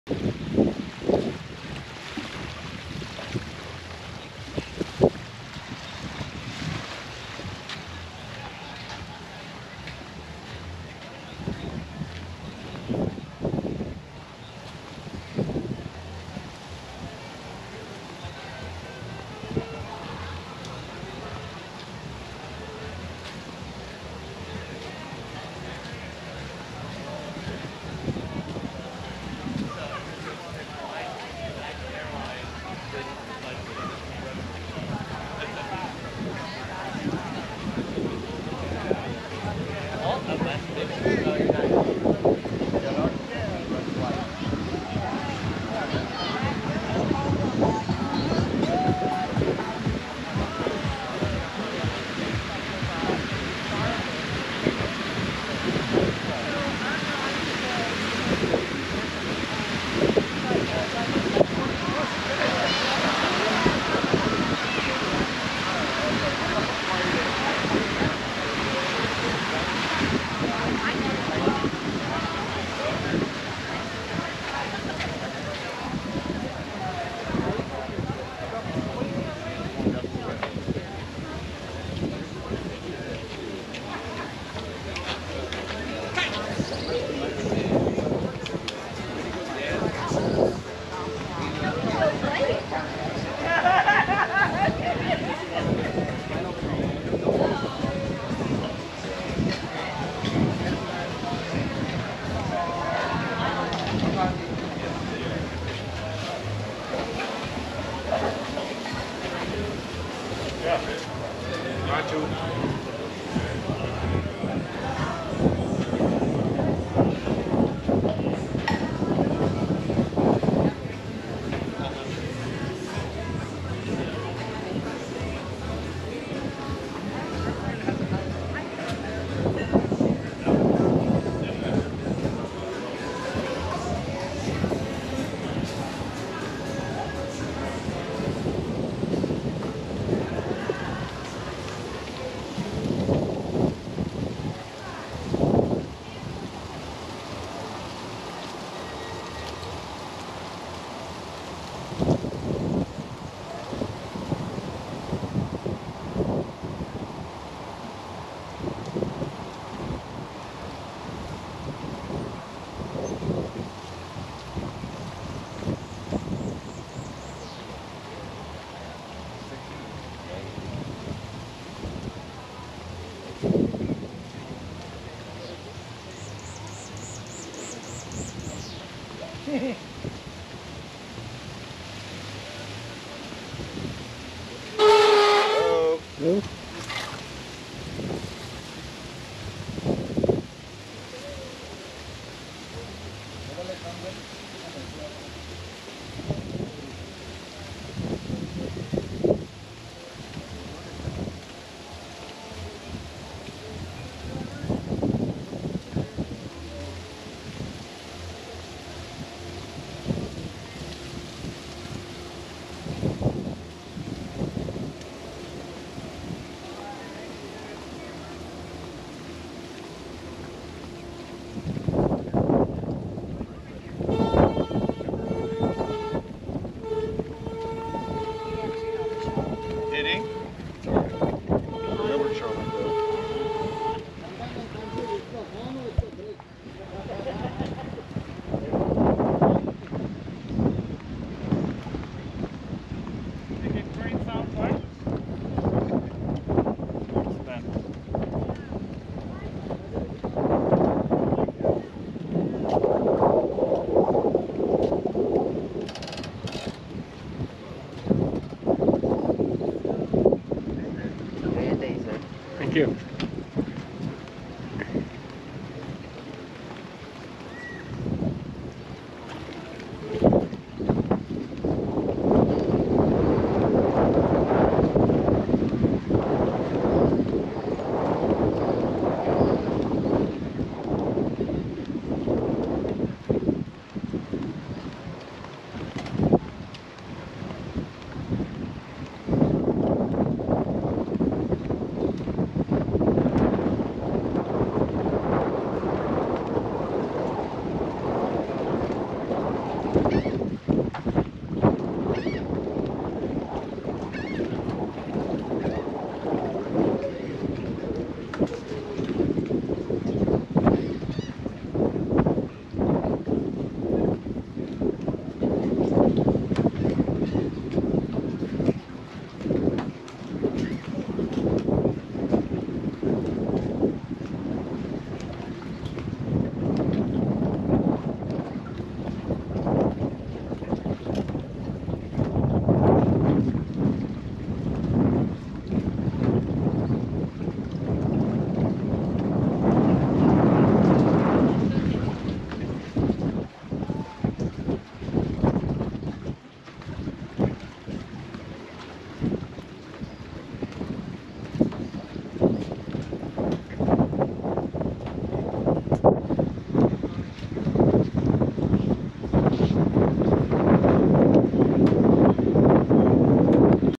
Coco Beach Club and Floating Cabanas on Royal Caribbean's CocoCay
A walkthrough of the Coco Beach Club and floating cabanas on Royal Caribbean's private island in the Bahamas, Perfect Day at CocoCay. The only over the water cabanas in the Bahamas.